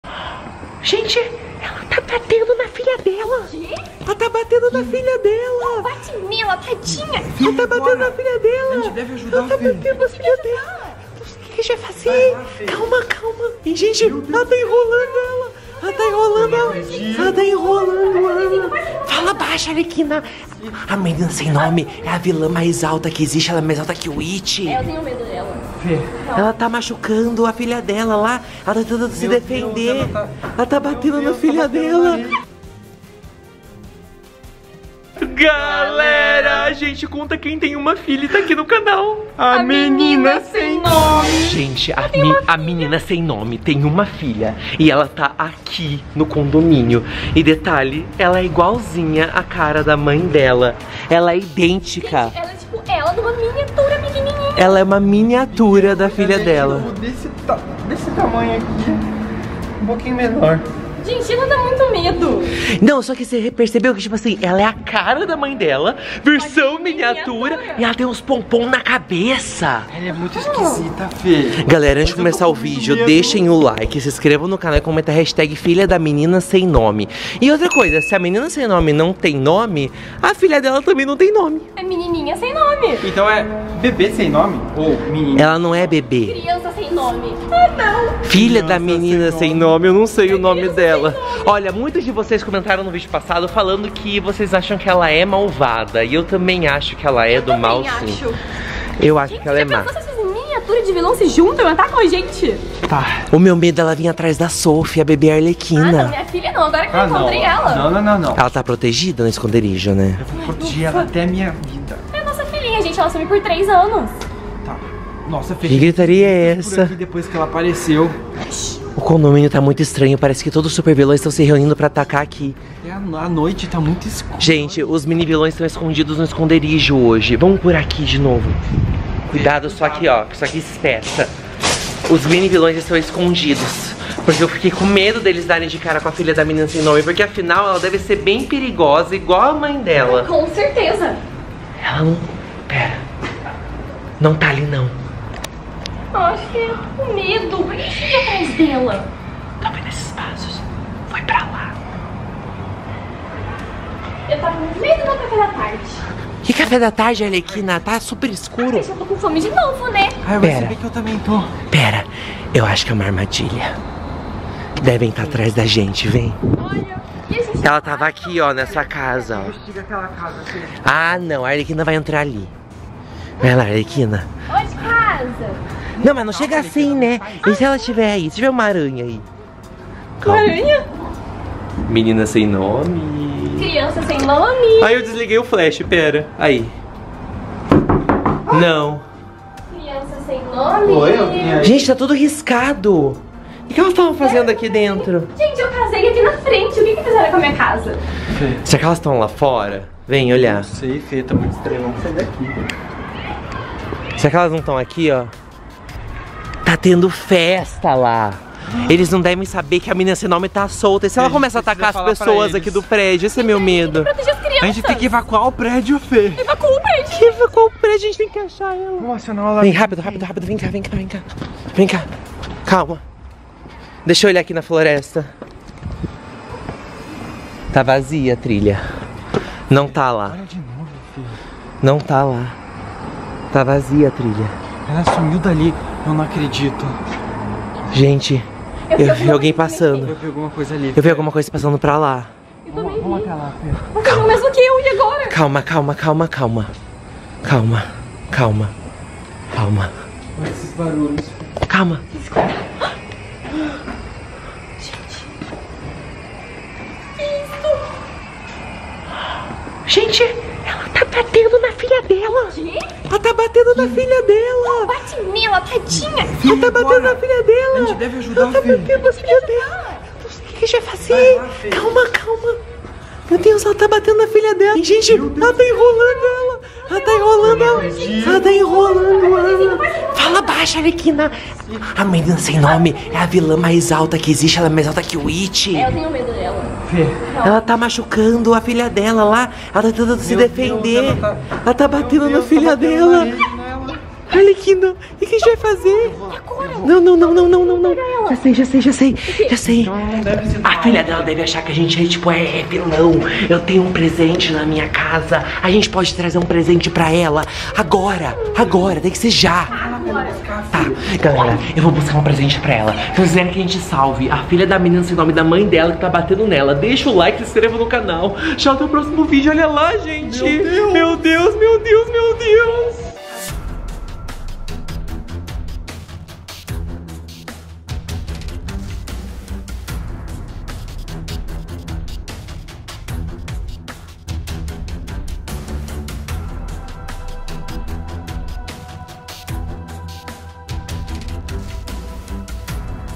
A MENINA SEM NOME BATEU NA SUA FILHA A FILHA DA MENINA SEM NOME ESTÁ EM PERIGO !!!
NESSE VÍDEO A MENINA SEM NOME BATEU NA SUA FILHA E O FELIPE, CORINGA E ARLEQUINA PRECISAM SALVA-LÁ !!!

CEP: 80410981
Curitiba Paraná